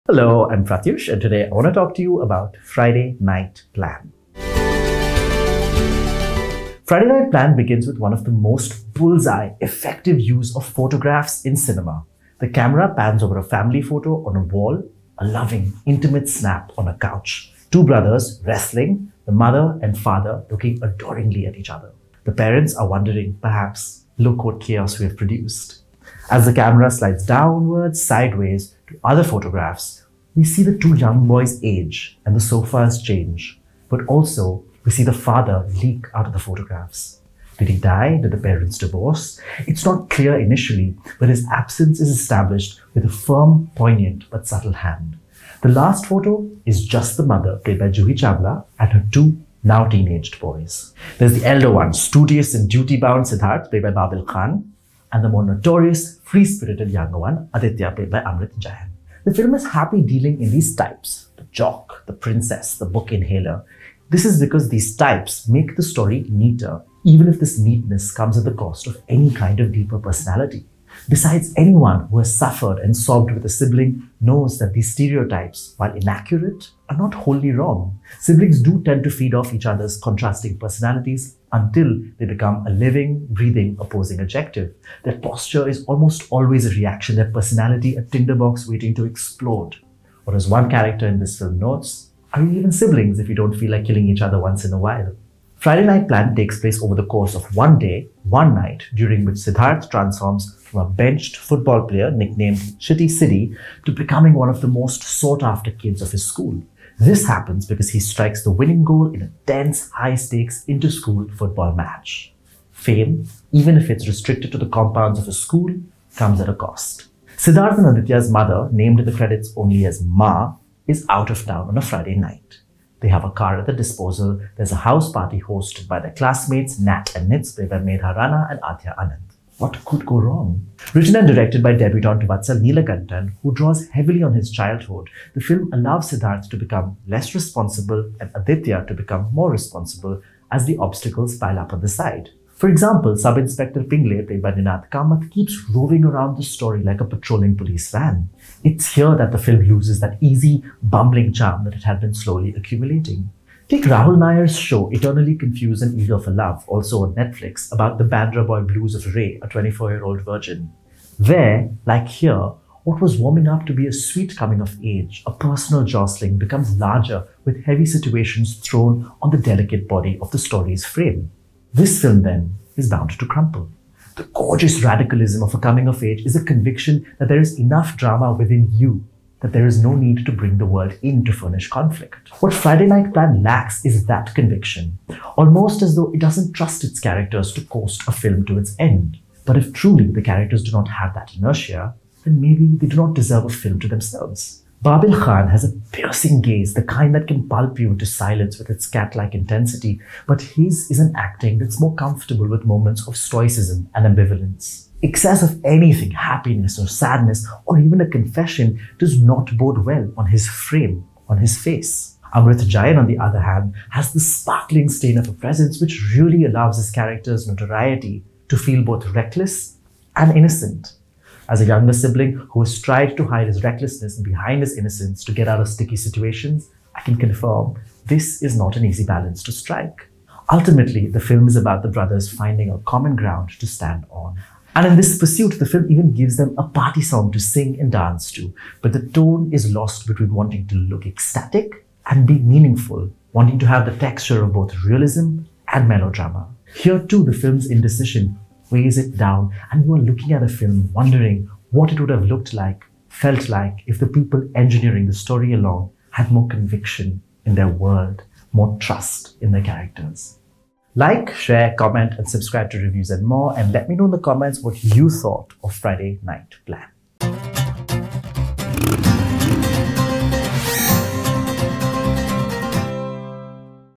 Friday Night Plan Movie Review by Prathyush | Babil Khan, Juhi Chawla | Film Companion
The film Friday Night Plan starring Juhi Chawla Mehta & Babil Khan revolves around brothers trying to find common ground. While the film offers them a party song to sing and dance to, its tone wavers—attempting to balance both ecstasy and depth, and straddling the line between realism and melodrama.